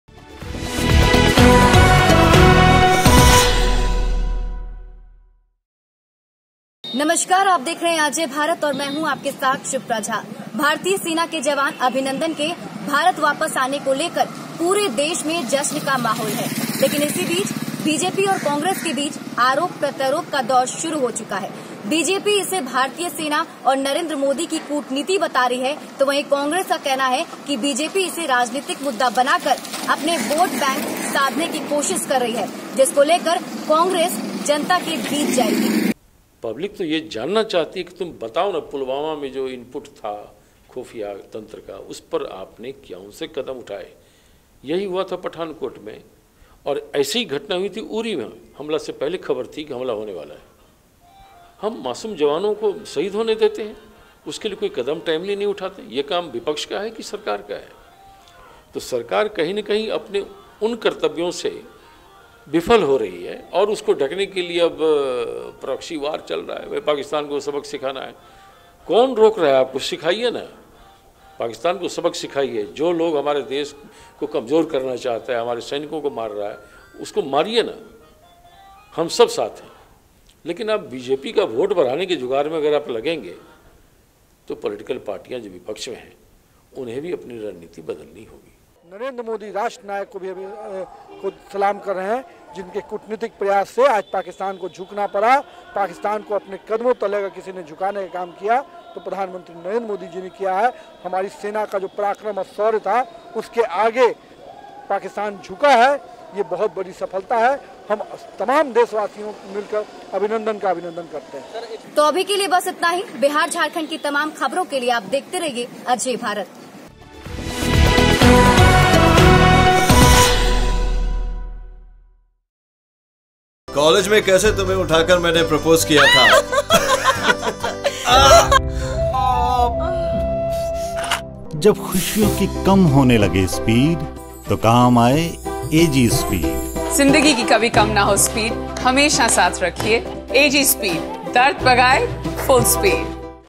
Abhinandan की रिहाई को लेकर Bihar में तर्क-कुतर्क शुरू, Bjp-Congress आमने-सामने
indiancaptain abhinandan की रिहाई को लेकर bihar में politics शुरू हो चुकी है। bjp और congress के बीच बयानबाजी के बीच किसने क्या क्या आइये आपको सुनाते हैं...